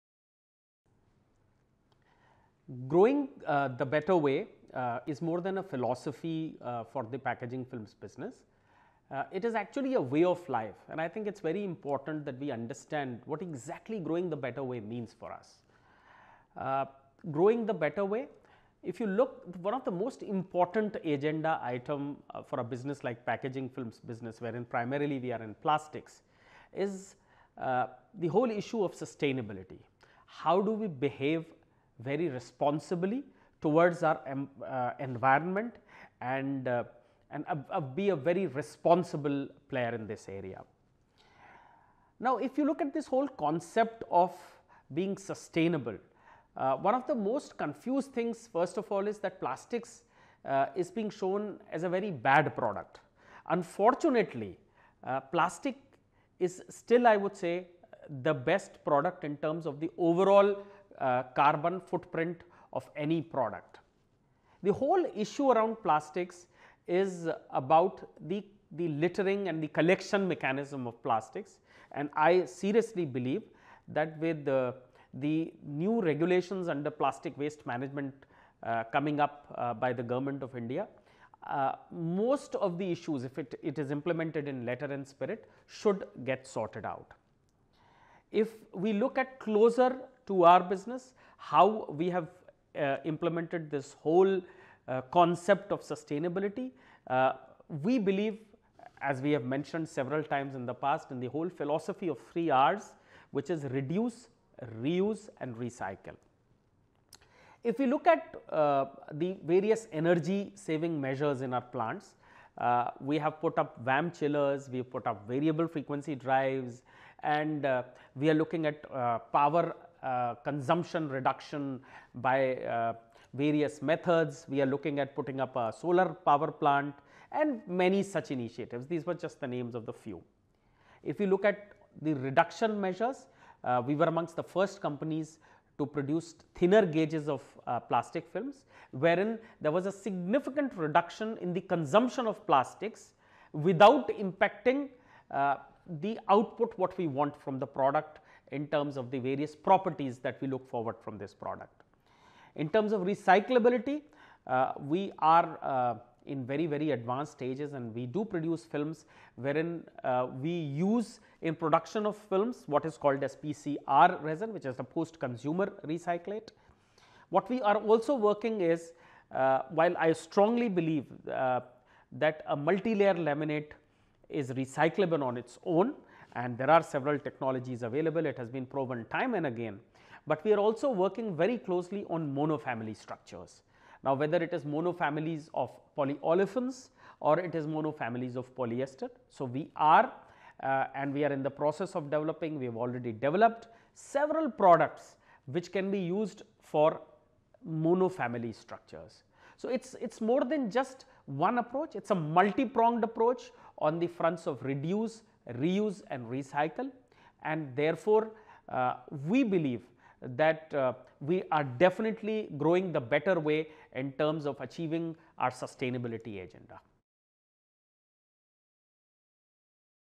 GrowingTheBetterWay at SRF
Did you know that plastics leave the least amount of carbon footprint as compared to any other packaging product? So where does the problem lie? Watch Prashant Mehra, President & CEO - Packaging Films Business, Coated & Laminated Fabrics Business untangle the myth created around plastics.